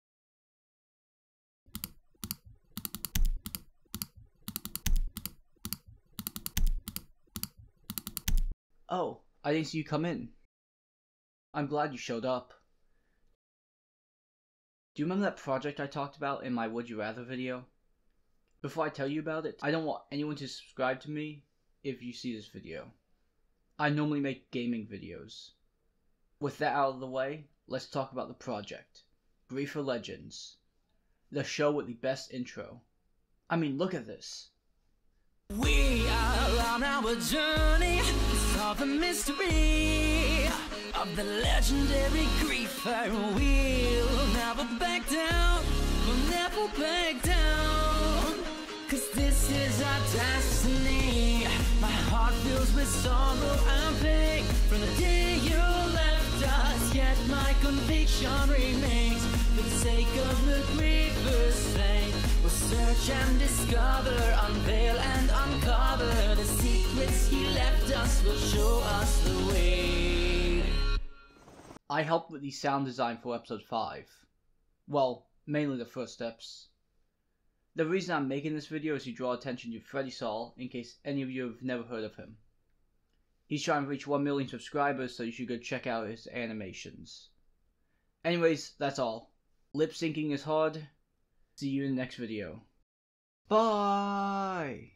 The project I worked on
Today I made an animation vlog thing to talk about the project I worked on. The animation was just for fun, wanted to try something different so if it sucks that's why.